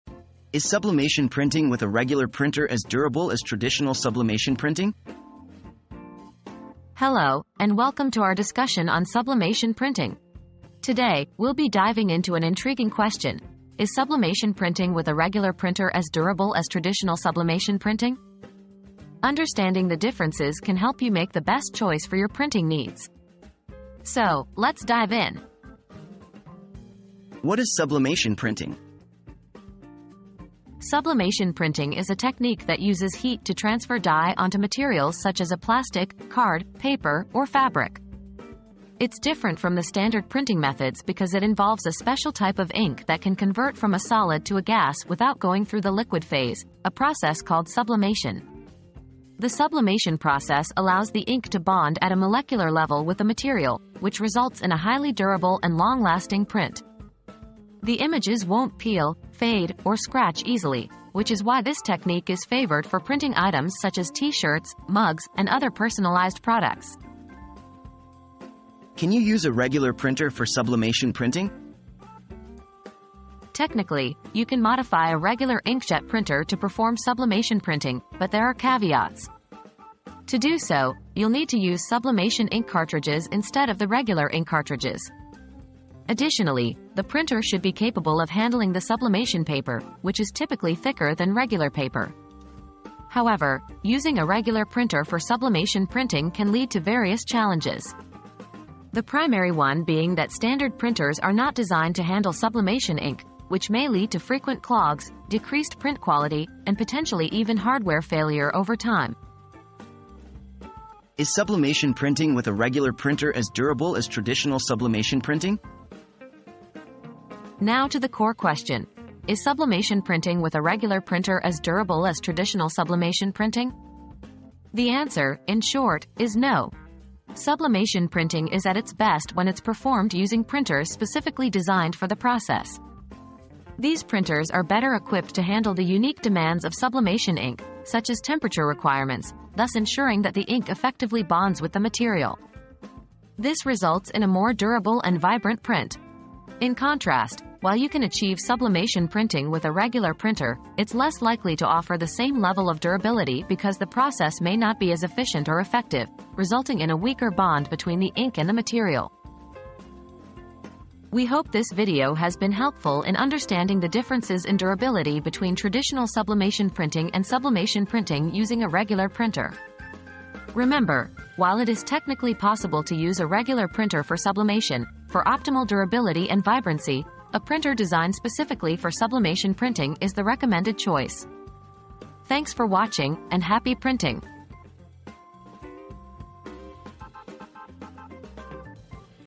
Is sublimation printing with a regular printer as durable as traditional sublimation printing?
Understanding Sublimation Printing Durability: Traditional vs Regular Printer • Curious about sublimation printing durability? Discover the differences between traditional and regular printer sublimation printing in this must-watch video! Can a regular printer deliver the same long-lasting, high-quality prints? Find out now!

00:00 • Is sublimation printing with a regular printer as durable as traditional sublimation printing?
00:27 • What is Sublimation Printing?
01:08 • Can You Use a Regular Printer for Sublimation Printing?
01:51 • Is Sublimation Printing with a Regular Printer as Durable as Traditional Sublimation Printing?